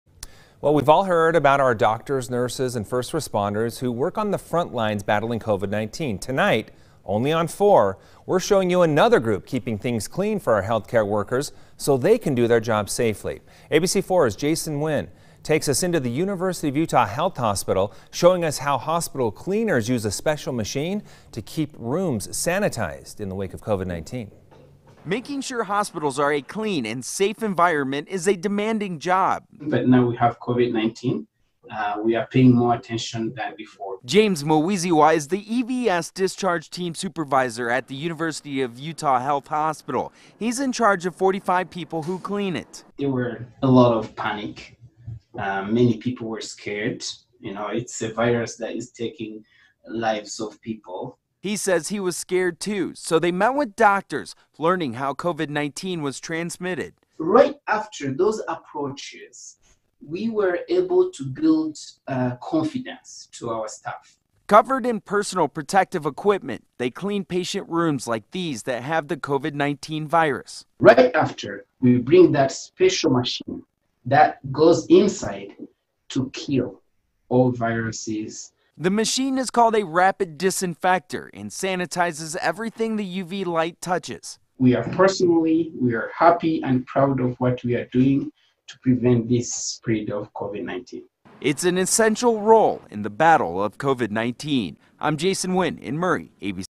Cleaning out COVID-19 at U of U Health
You've heard about our doctors, nurses, and first responders who work the front lines battling COVID-19. One group of people who are often overlooked are the ones who ensure they can do their jobs, the hospital cleaners.